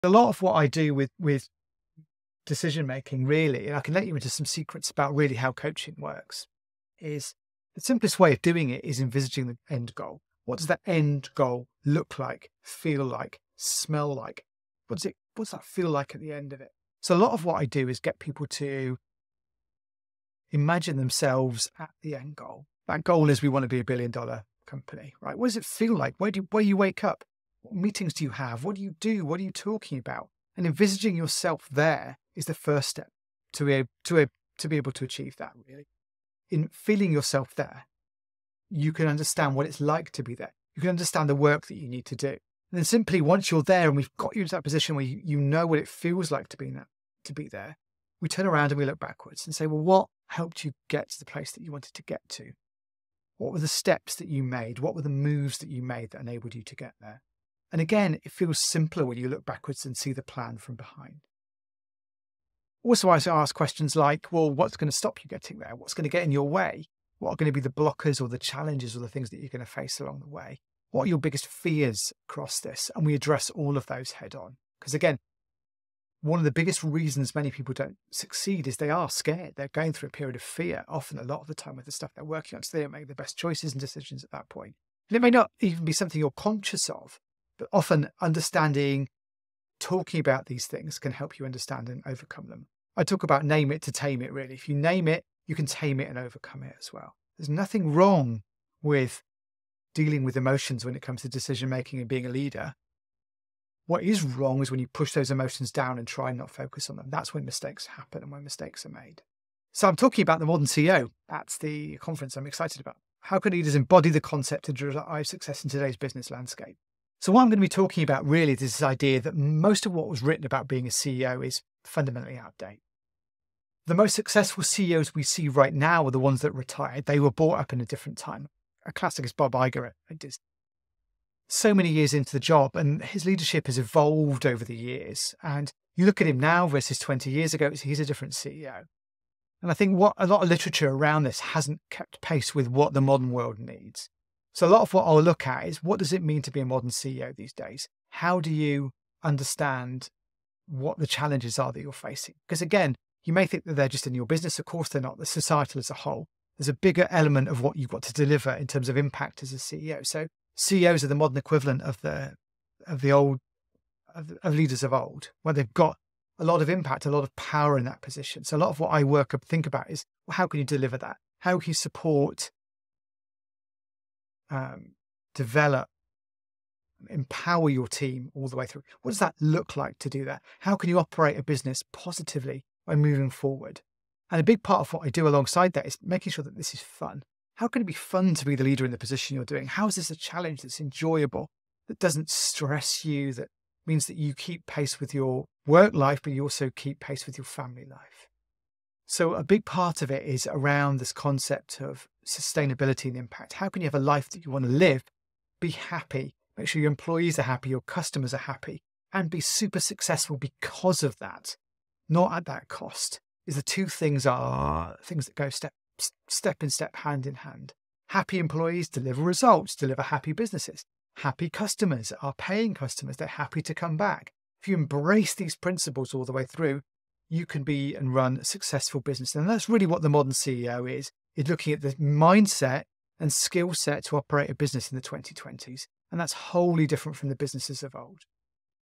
Making Better Decisions | MrJoe - Coach to CEOs
A brief dive into just one of the topics I'll be talking about on stage at @HowToWebConf in October.

If you'd like to have a look at the agenda and book tickets, you can get them

If you're already going, I'll look forward to seeing you there!

If you enjoy my content and you want to get in touch to find out how I could work with you, or someone you know, you can reach me on LinkedIn or via email at: !